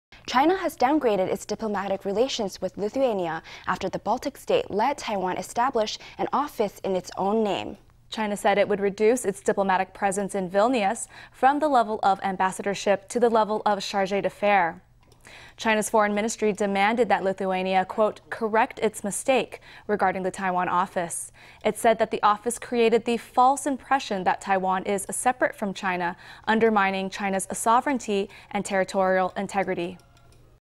Beijing downgrades ties with Lithuania after Taiwan sets up embassy in Vilnius
China has downgraded its diplomatic relations with Lithuania, after the Baltic state let Taiwan establish an office in its own name. China said it would reduce its diplomatic presence in Vilnius, from the level of ambassadorship to the level of charge d’affaires.

China’s foreign ministry demanded that Lithuania “correct its mistake” regarding the Taiwan office. It said that the office created the false impression that Taiwan is separate from China, undermining China’s sovereignty and territorial integrity.